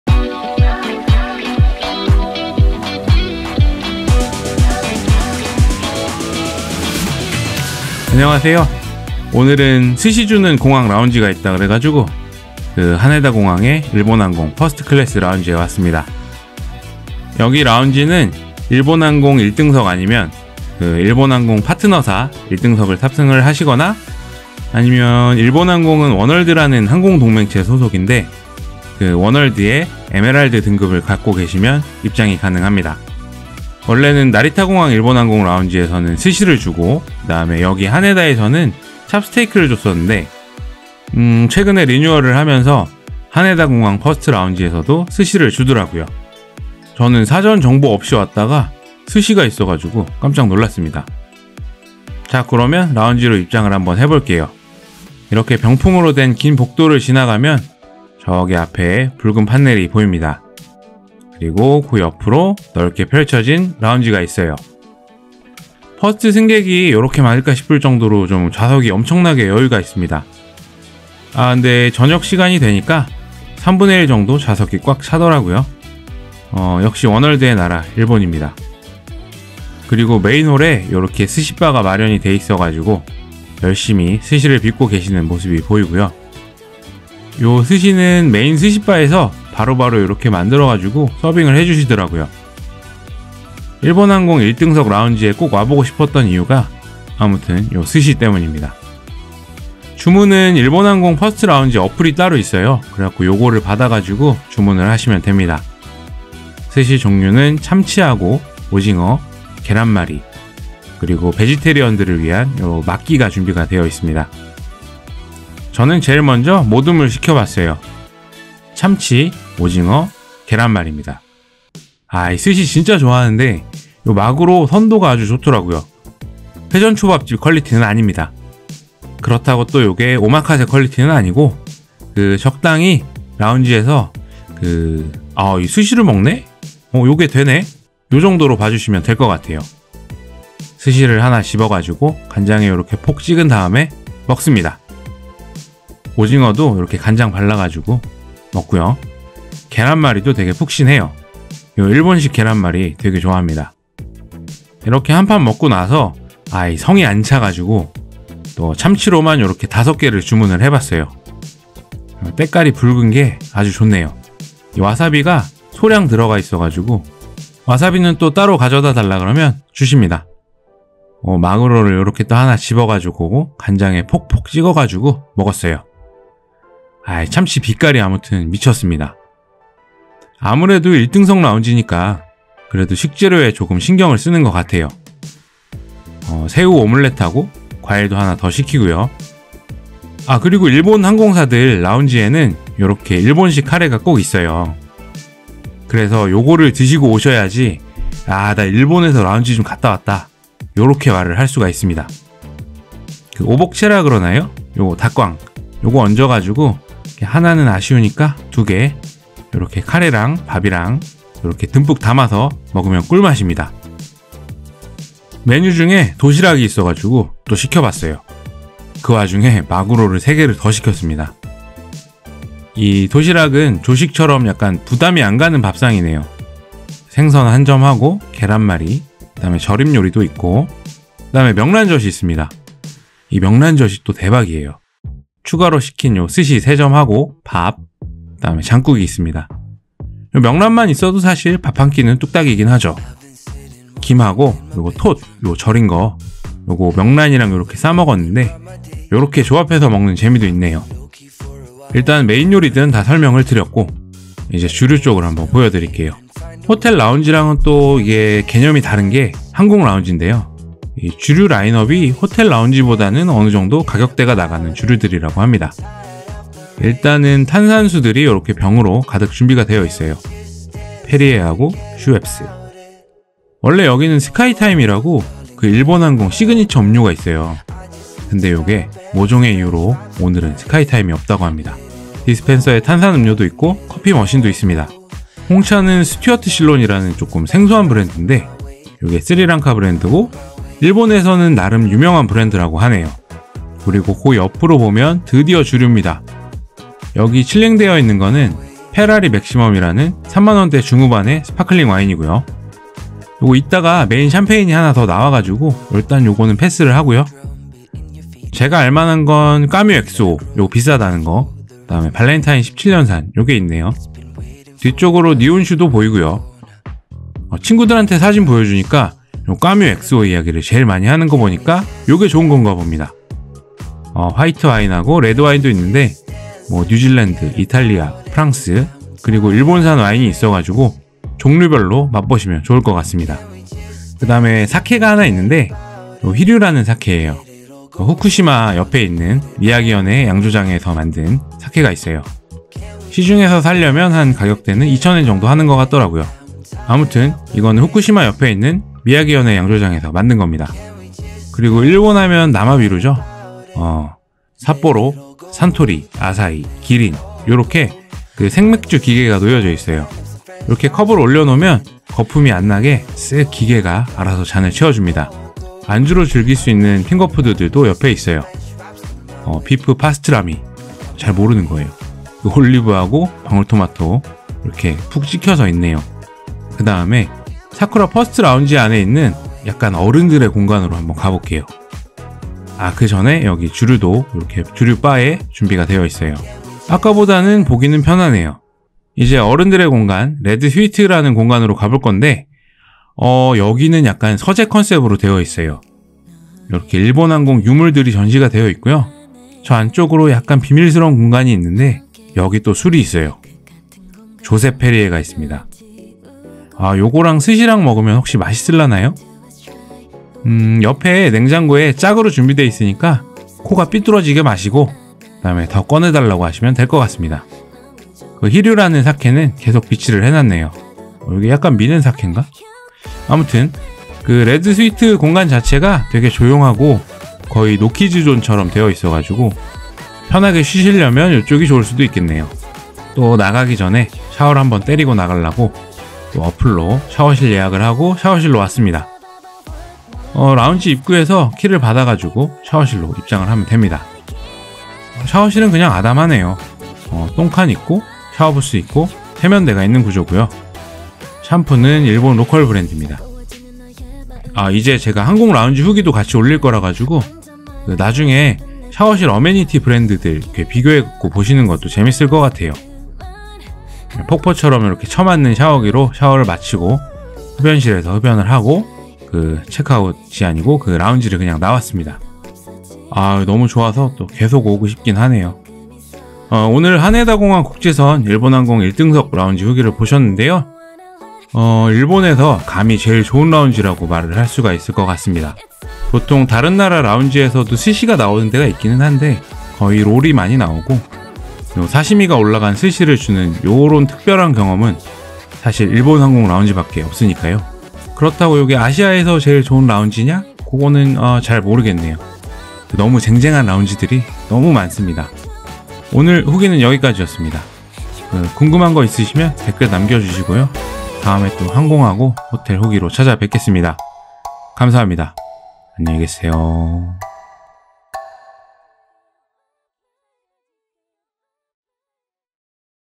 스시주는 공항 라운지, 하네다 일본항공 일등석 라운지 후기 l 하네다 공항에 스시를 만들어 주는 항공사 라운지가 있다고 해서 다녀왔습니다, 일본항공 퍼스트클래스 라운지 후기
스시를 자리에서 만들어주는 공항 라운지!
일본항공 사쿠라 퍼스트 클래스 라운지에 다녀왔습니다
감히 일본에서 제일 좋은 라운지라고 소개 할 만한 라운지!

스시도 있고, 벤또도 있고 라운지에서 고급음식을 즐길 수 있는
하네다공항 일본항공 퍼스트라운지! 함께 보시져!